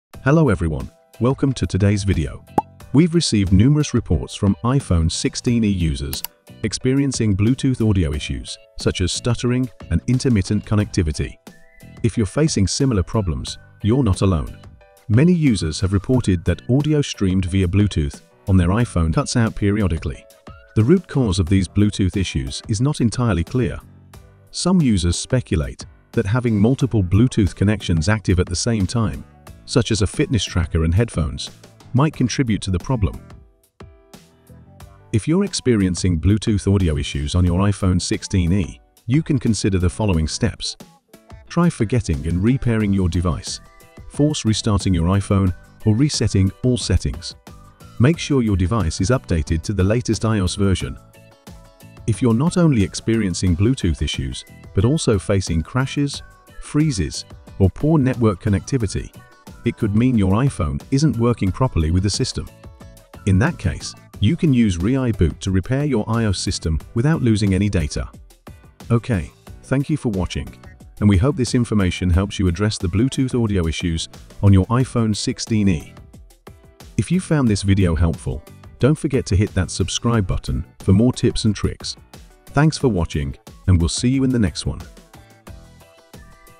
iPhone 16e Has a Bluetooth Audio Problem? Here’s How to Fix It!
Are you experiencing Bluetooth audio stuttering or connection issues on your iPhone 16e? Many users have reported similar problems, but don’t worry—we’ve got the solutions for you!  Watch this video for quick fixes to resolve Bluetooth audio problems on your iPhone.

*⏱TIMESTAMP:*
00:00 iPhone 16e Bluetooth Audio Problem
00:31 How to Fix it?
01:00 Repair iOS with ReiBoot

*How to Fix Bluetooth Audio Issues on iPhone 16e*

- *Method 1: Forget & Re-pair Bluetooth Device*
  - Go to Settings - Bluetooth.
  - Tap the (i) icon next to your device and select "Forget This Device."
  - Reconnect your Bluetooth device.

- *Method 2: Force Restart Your iPhone*
  - Press and release the Volume Up button.
  - Press and release the Volume Down button.
  - Hold the Side button until the Apple logo appears.

- *Method 3: Reset All Settings*
  - Go to Settings - General - Transfer or Reset iPhone - Reset.
  - Tap “Reset All Settings” (this won’t delete your data).

- *Method 4: Update to the Latest iOS*
  - Go to Settings - General - Software Update.
  - Download and install any available updates.

- *Method 5: Repair iOS with ReiBoot*
  - Download and install ReiBoot on your computer.
  - Connect your iPhone and select "Standard Repair" to fix system issues.
  - Download the firmware and repair your iPhone without data loss.